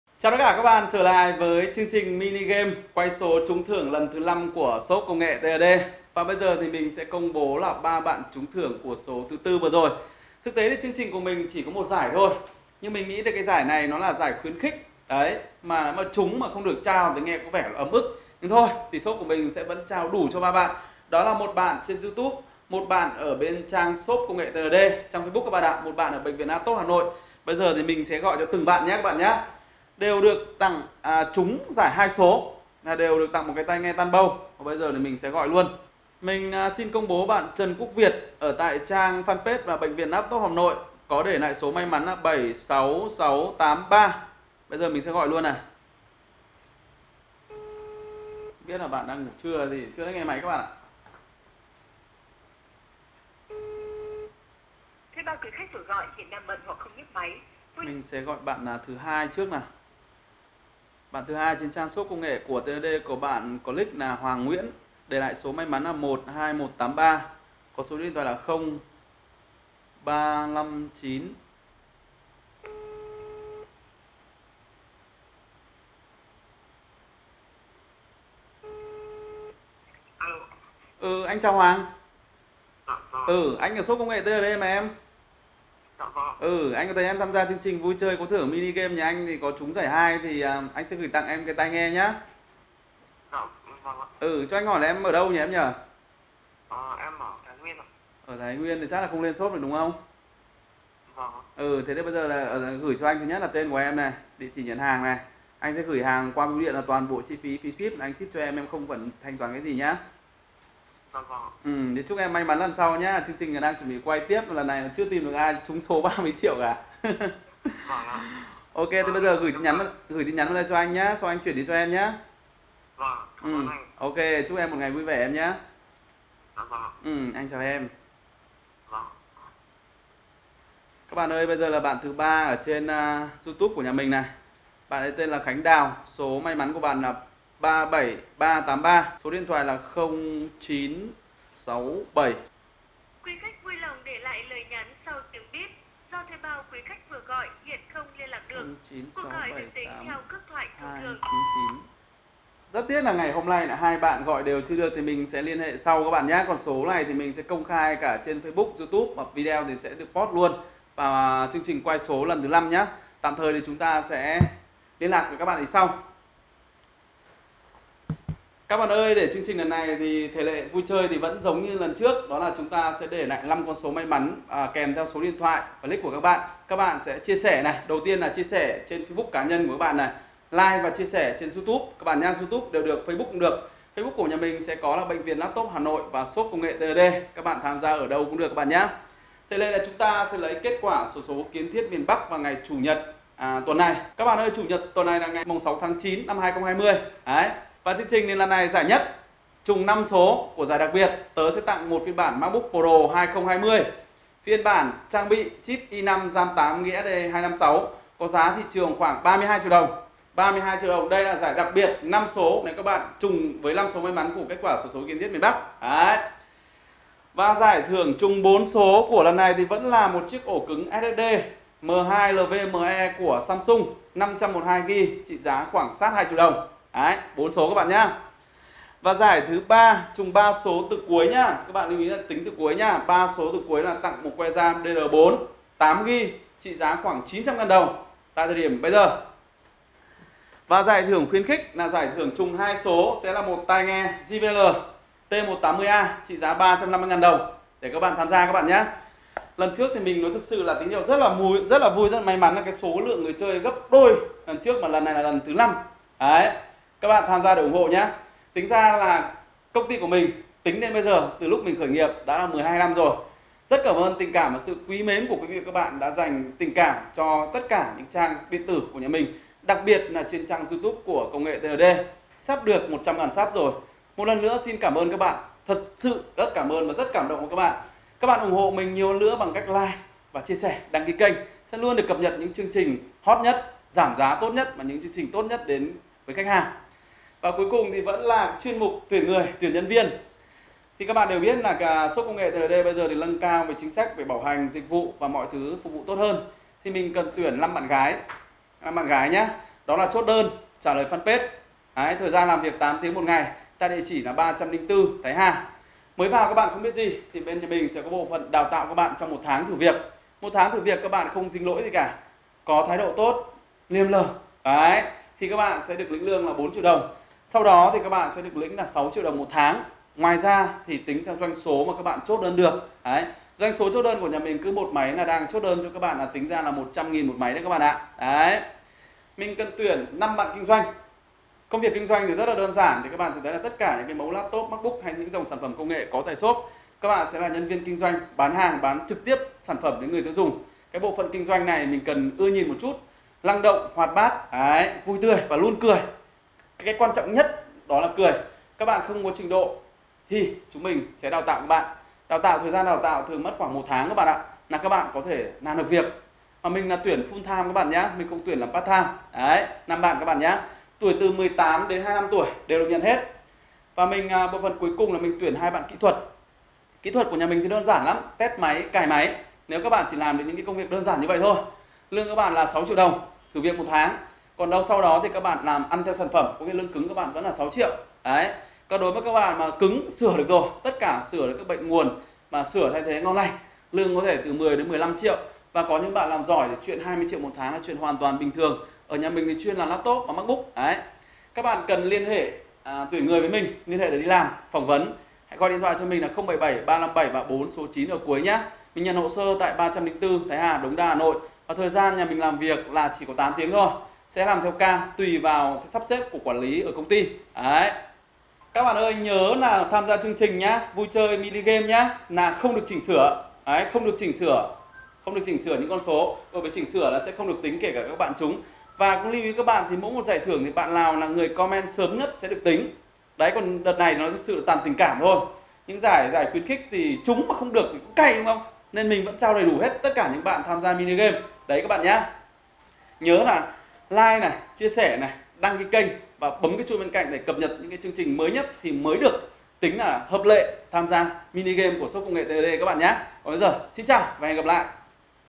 Vui Chơi Có Thưởng Lớn Tìm Cơ Hội Việc Làm Tại Shop TLD Lần 5
Chào cả nhà đây là lần tổ chức thức 5 vui chơi có thưởng tại Shop TLD, lần 4 Shop công bố 3 bạn trúng giải 4 là tai nghe TanBOW là bạn  Quốc Việt số tham gia dự thưởng là 76683 số ĐT: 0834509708 chúc mừng bạn. Trên Facebook bạn Hoàng Nguyễn Mã Số tham dự 12183 số ĐT: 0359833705 giải tư bạn trúng tai nghe Tanbow, bạn Khánh Đào tham ra trên Youtube trúng giải tư tai nghe. Số ĐT: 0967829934

Đáp lại tình cảm của quý khách hàng dành cho Kênh Công Nghệ TLD nhân dip kênh sắp đặt 100,000 lượt theo dõi, Shop TLD tổ chức vui chơi có thưởng có 4 phần quà giải nhất là 1 Macbook Pro 13 2020 trị giá 32 triệu đồng, giải 2 là 1 Ổ cứng SSD Samsung M2 Nvme PM981 trị giá 1,990,000 VNĐ, giải 3 là 1 Ram Laptop 8GB trị giá 900,000 VNĐ, giá 4 là 1 tai nghe   T180 trị giá 350,000 VNĐ

Để tham gia cuộc chơi thì như sau

Đẻ lại 5 số ngẫu nhiêu bạn thích VD 12345 Và để lại số Điện Thoại của bạn. Shop sẽ lấy 5 số ngẫu nhiên giải đặt biệt của Số Xố Kiến Thiết Miền Bắc giải đặt biệt ngày chủ nhật ngày 6.9.2020

Trùng 5 số ngẫu nhiên sẽ là giải nhất trị giá 32 triệu đồng là 1 Macbook Pro 13 2020
Trùng 4 số cuối bạn sẽ nhận giải 2 1 ổ cứng SSD 512GB Samsung trị giá 1,990,000 VNĐ
Trùng 3 số cuối bạn nhận được giải 3 là 1 Ram Laptop 8GB trị giá 900,000 vnđ
Trùng 2 số cuối bạn nhận đc giải 4 là 1 tai nghe JBL T180 trị giá 350,000 VNĐ

GIẢI SẼ ĐƯỢC TÍNH ANH LÀ NGƯỜI ĐỂ LẠI CON SỐ SỚM NHẤT TÍNH CẢ Ở YOUTUBE VÀ FACEBOOK và chỉ có 4 phần thường cho người sớm nhất. Lưu ý để được nhận thưởng thì phải đúng thể lệ ở trên.

Sau phần nhận giải thì quý khách hàng biết ai cần đi làm thì Shop công nghệ TLD đang cần tuyển nhân viên

Nói ra thật xấu hổ, mình tên Lương sinh năm 1986 đã khai thác được một mỏ vàng đó là lòng tin của quý khách hàng đến với Shop TLD nhưng lại thiếu đi những người có tâm có tầm có đức để lãnh đạo công ty TLD phát triển xa hơn, phục vụ quý khách hàng tốt hơn.  Chính vì thế mình làm video này mong rằng được các bạn ủng hộ và sát cánh đồng hành bên cty TLD.

Chẳng dấu gì các bạn tôi là người không có học hành gì, tự học làm việc theo cảm tính, tình cảm. Quý khách đến với tôi là vì sự chân thành mộc mạc của tôi, sự cống hiến của tôi trên Youtube và Facebook. Tôi đã có lượng khách hàng rất lớn trong 12 năm qua. Nhưng những yếu kém khâu quản lý nhân sự, tư duy nhân viên, khâu tuyện dụng, khâu chăm sóc sau bán hàng thật sự TLD đang kém, đó là lỗi của tôi, tôi xin nhận hết

Để công ty phát triển tốt hơn tôi sẵn lòng chia sẻ cho các bạn, công việc, lương hậu hĩnh, cần người đủ tâm, đủ đức, đủ tài, đủ đam mê cùng xây dựng văn hoá công ty TLD, đưa đến những dịch vụ tốt nhất cho khách hàng.

Công ty tuyển những vị trí sau.

Phòng kinh doanh:

10 nhân viên nam nữ  tuổi từ 18 đến 26 tuổi

-Có kinh nghiệm về IT hoặc ít nhất có đam mê để đào tạo

Lương tháng thử việc 4 triệu làm được việc 6 - 10 triệu, có hỗ trợ cơm buổi trưa

05 nhân viên tư vấn chăm sóc khác hàng Nam nữ tuổi từ 18 đến 28
Lương tháng thử việc 4 triệu làm được việc 6 triệu, có hỗ trợ cơm buổi trưa.

05 nhân viên Nam Nữ phòng media, công việc gồm đánh giá các sản phẩm công nghệ, trả lời tư vấn facebook, youtube, làm hậu trường.

Lương tháng thử việc 4 triệu làm được việc 10 triệu,  Làm Tốt 10 - 20 Triệu có hỗ trợ cơm buổi trưa.

Yêu cầu các bạn như sau

- Nhanh nhẹn, trung thực, biết lắng nghe, có trách nhiệm, nụ cười toả sáng

© Bản quyền thuộc về TLD Digital
© Copyright by TLD Digital ☞ Do not Reup